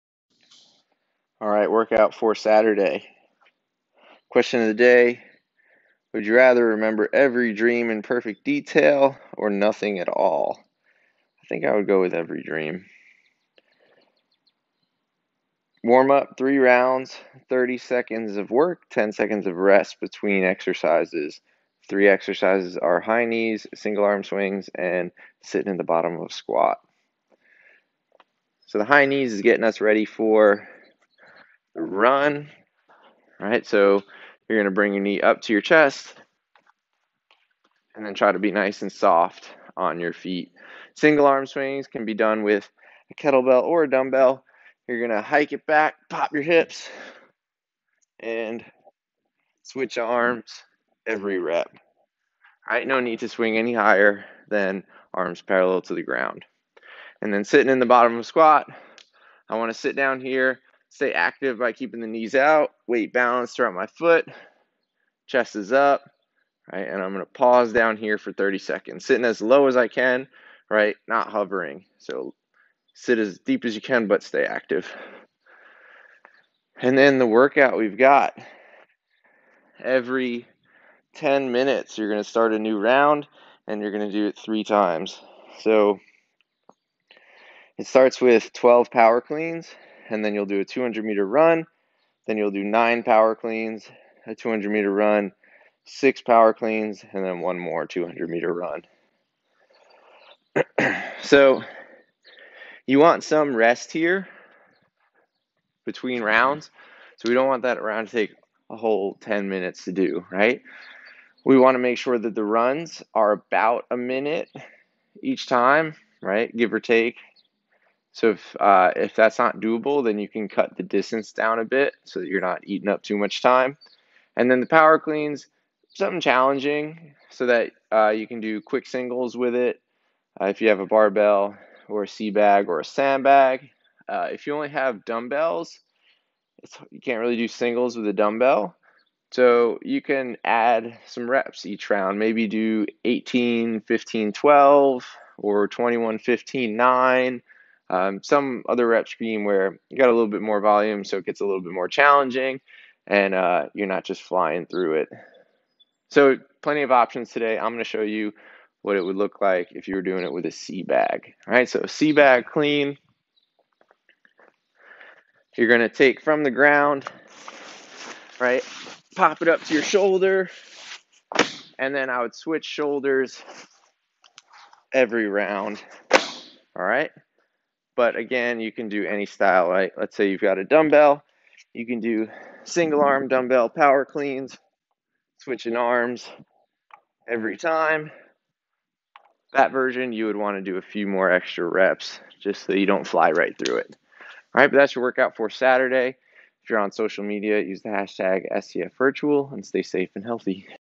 At home workout - power cleans, run - 20200510
Warm up
3 rounds - :30 on / :10 off
High knees
Single arm swings
Bottom of squat

Conditioning
Every 10:00 for 3 rounds
12 power cleans
200m run
9 power cleans
200m run
6 power cleans
200m run